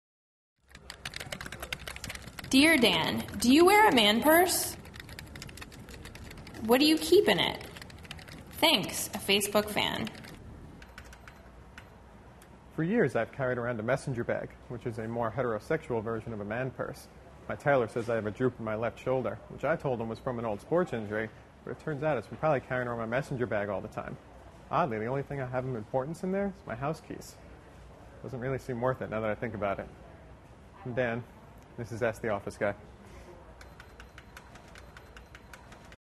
Do You Carry a Man Purse?
Dan manages to keep his personal life very, well, personal. But every so often he gives us a revealing glimpse into his world. In this Ask the Office Guy, he tells us whether or not he wears a man purse or, more familiarly, murse.